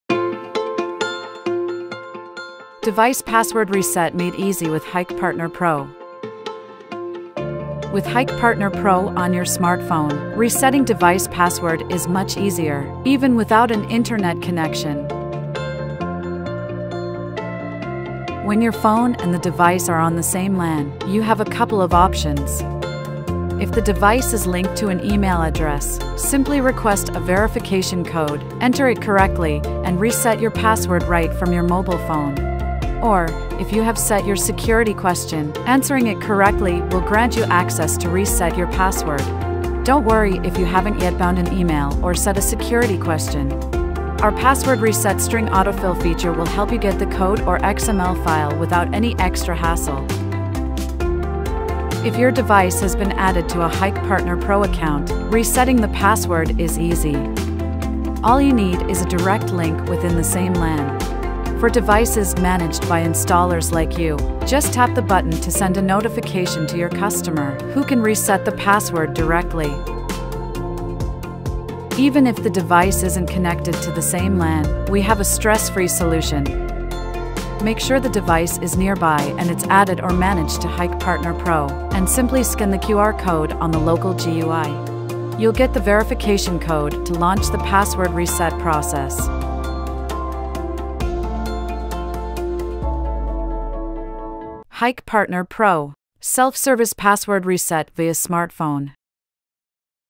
Device Password Reset Made Easy with Hik Partner Pro
Hikpartner Pro resetting device password made very easy.